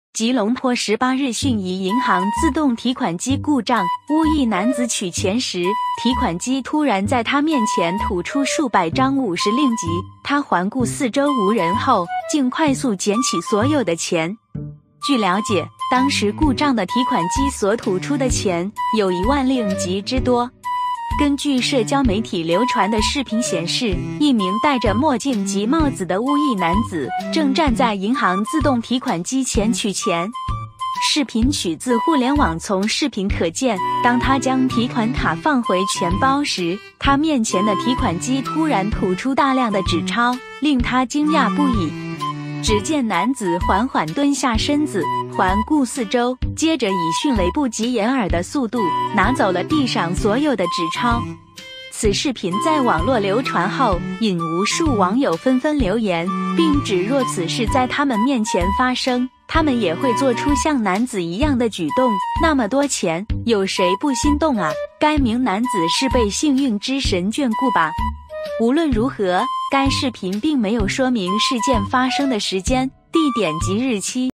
自动提款机疑故障 按钱它吐钱 男子看没人快快Sapu
（吉隆坡18日讯）疑银行自动提款机故障，巫裔男子取钱时，提款机突然在他面前“吐“出数百张50令吉，他环顾四周无人后，竟快速捡起所有的钱！

据了解，当时故障的提款机所“吐出”的钱，有1万令吉之多。

根据社交媒体流传的视频显示，一名戴着墨镜及帽子的巫裔男子，正站在银行自动提款机钱取钱。

从视频可见，当他将提款卡放回钱包时，他面前的提款机突然吐出大量的纸钞，令他惊讶不已。

只见男子缓缓蹲下身子，环顾四周，接着以迅雷不及掩耳的速度，拿走了地上所有的纸钞。

此视频在网络流传后，引无数网友纷纷留言，并指若此事在他们面前发生，他们也会做出像男子一样的举动，“那么多钱，有谁不心动啊”、“该名男子是被幸运之神眷顾吧”。

无论如何，该视频并没有说明事件发生的时间、地点及日期。